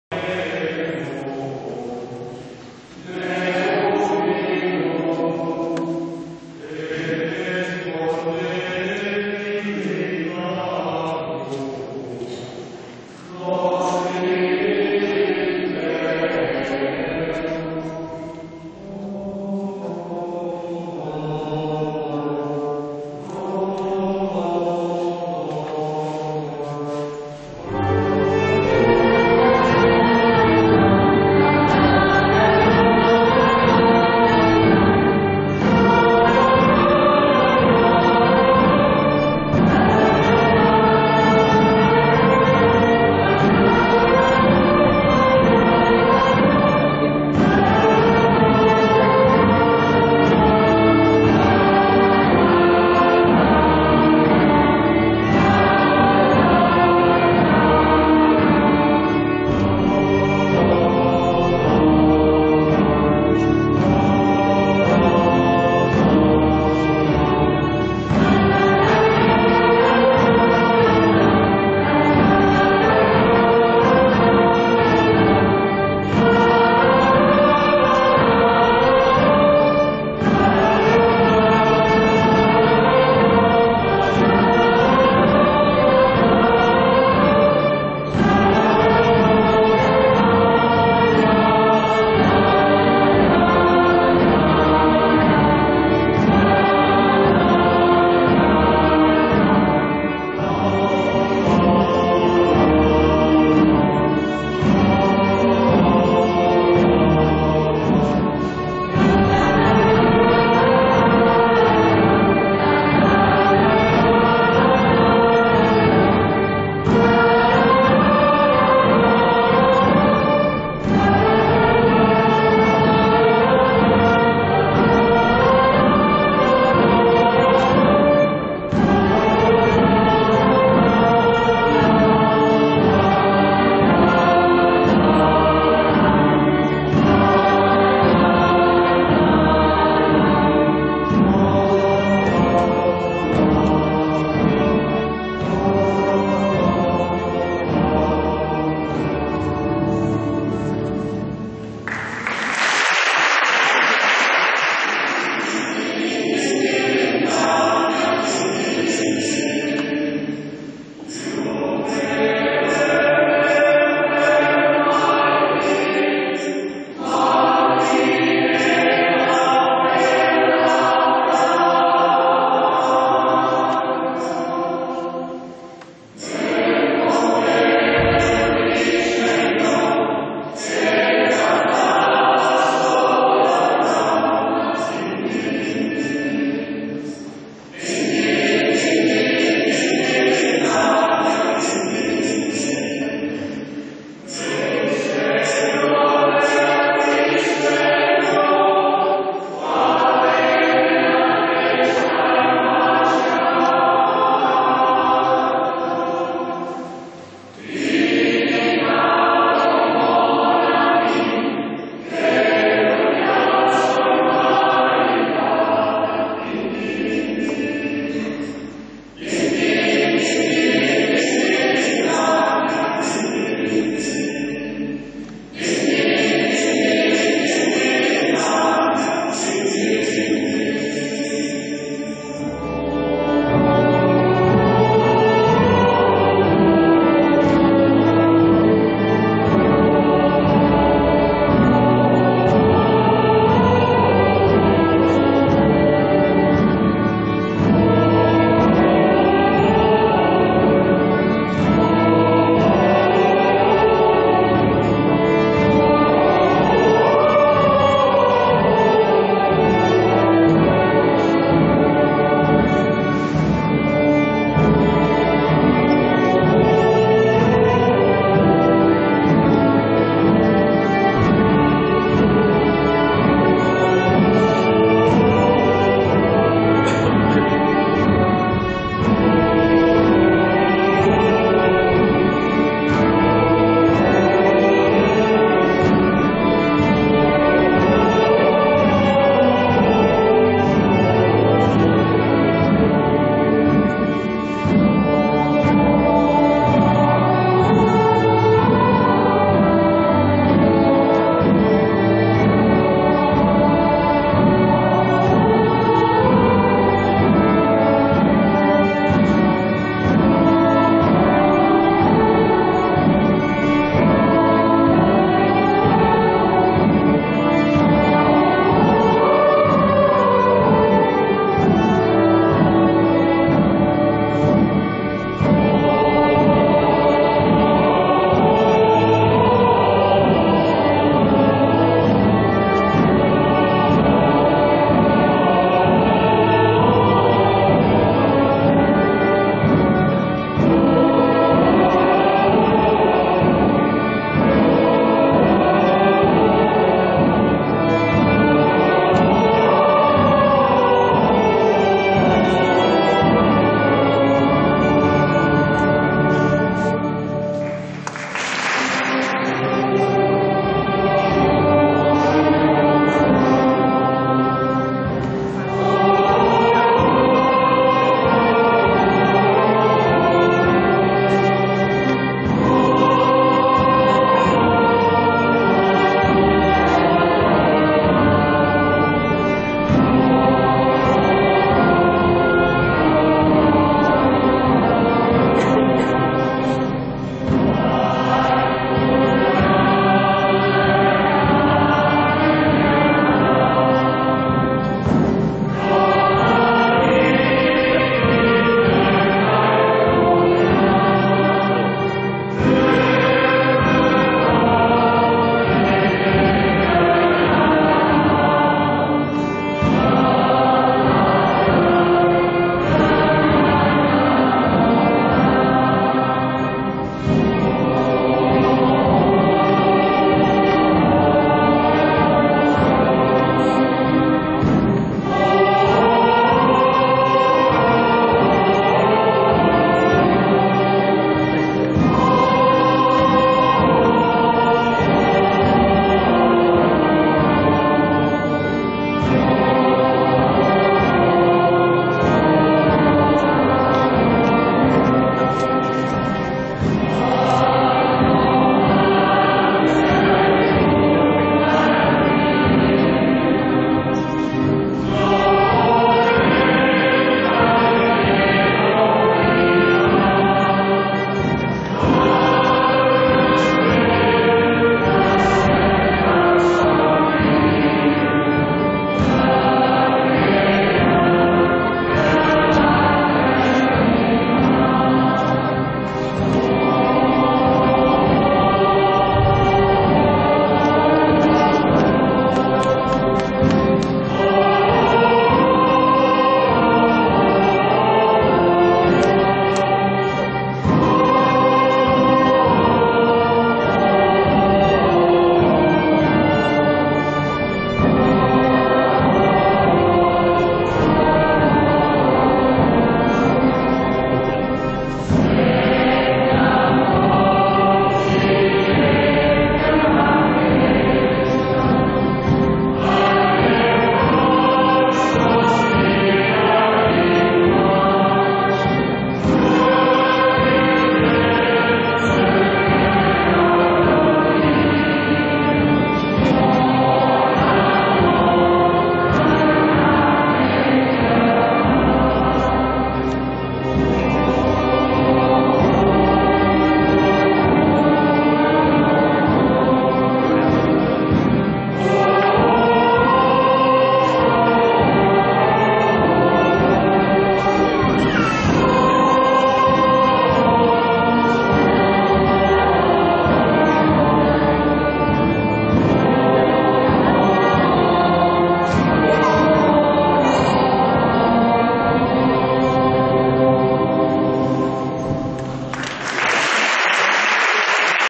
Concert i dansa medieval. Coral Gabellina i Escola de dansa Stylo
Direcció Elionor Gómez-Quintero i Claudia Suiter.
Concert de Cloenda del Mercat Medieval de Capdepera 2014
Església de Sant Bartomeu